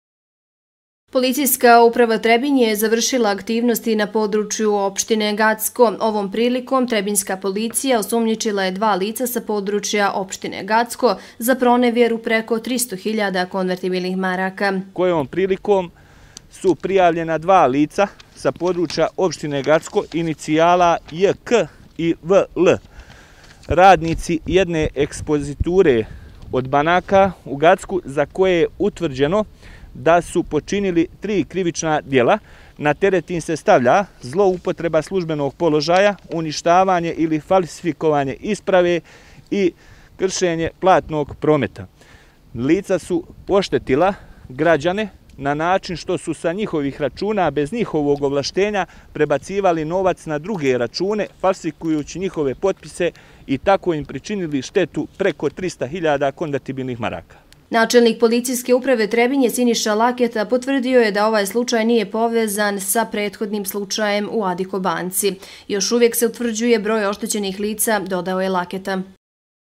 Radnici banke u Gacku pronevjerili više od 300.000 KM
Trebinjska policija osumnjičila je dva službenika jedne banke u Gacku za pronevjeru preko 300 hiljada КM. Riječ je o licima inicijala J.K. i V.L. Još nije uvtrđen tačan broj oštećenih lica.